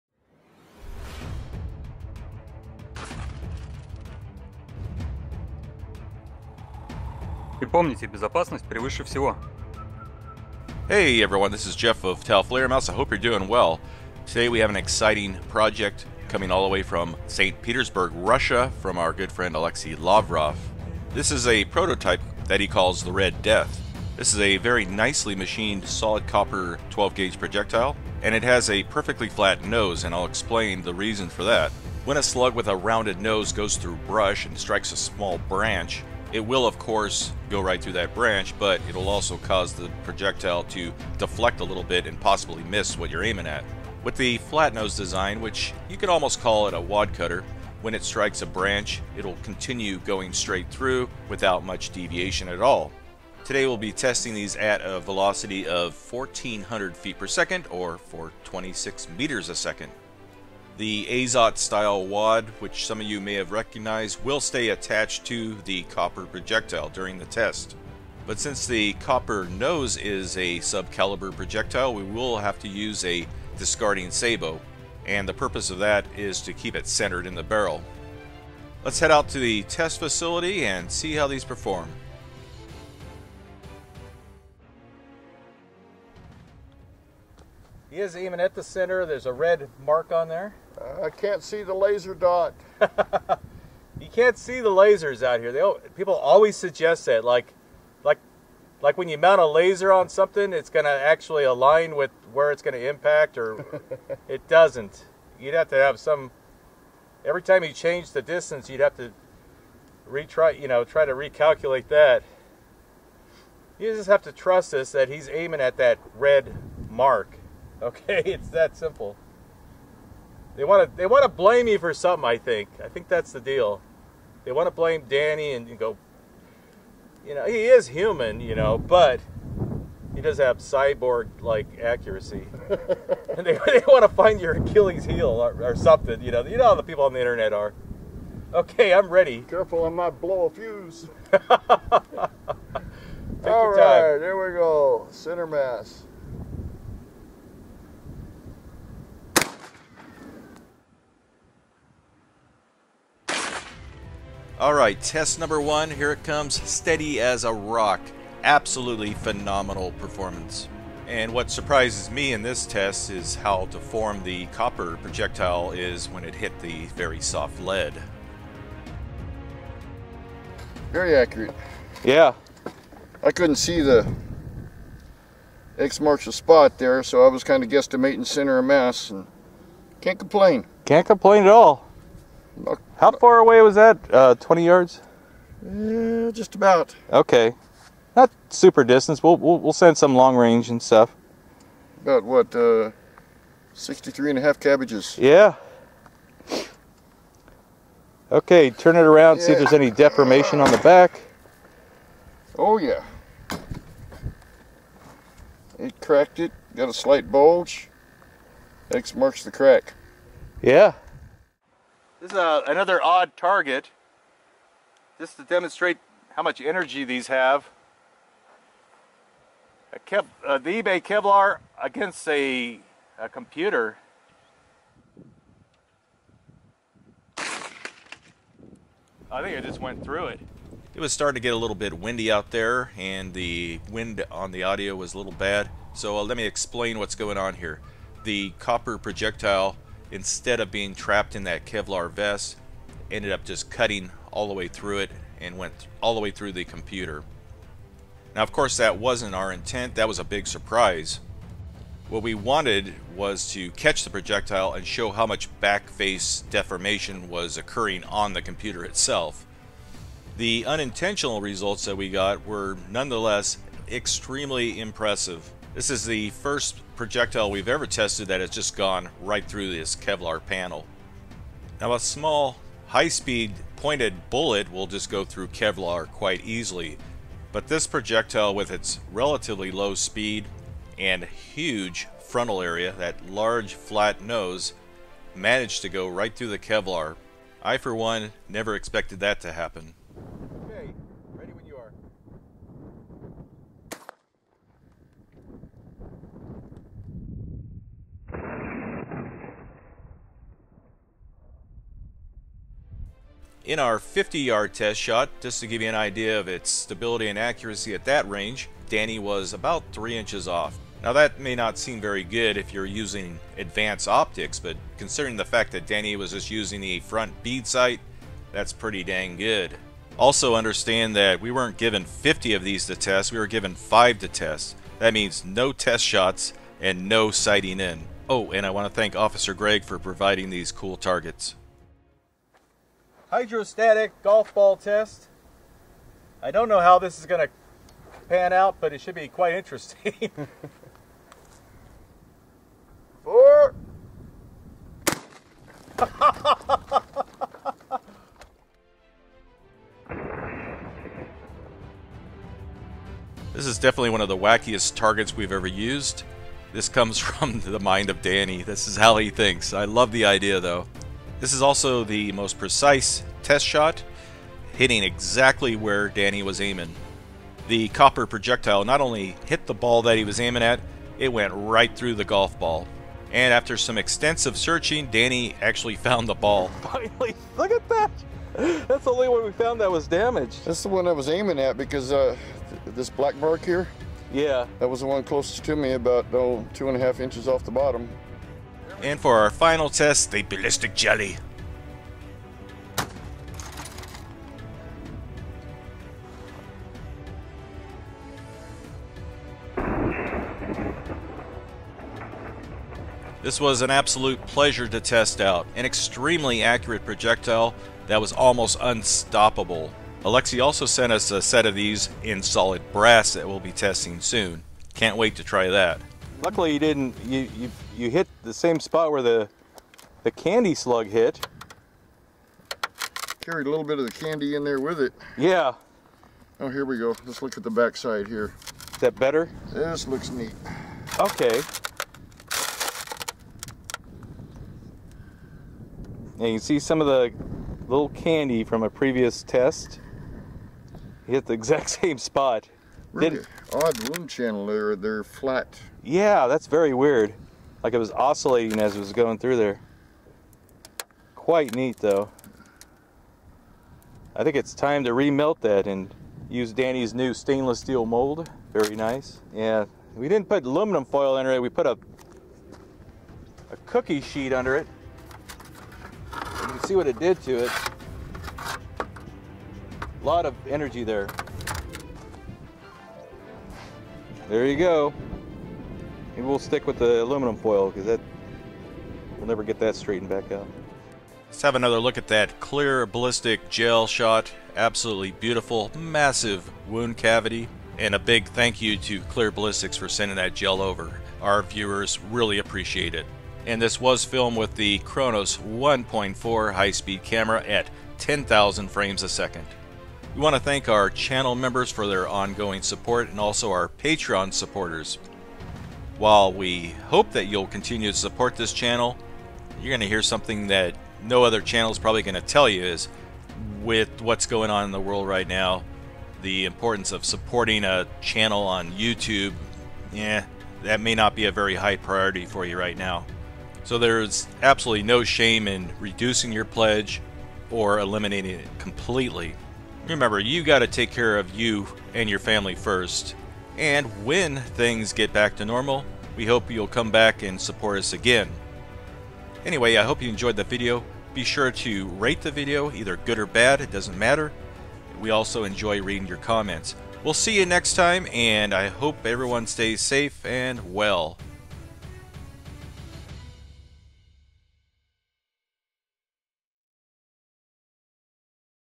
Prototype RUSSIAN 12g. slug is almost unstoppable!
There are NO firearms in this video,  YT reviewers.   In this video we test a new design by Alexey from Russia.   Alexey tests his designs using computer modeling/dynamics and we verify his results using a high speed camera.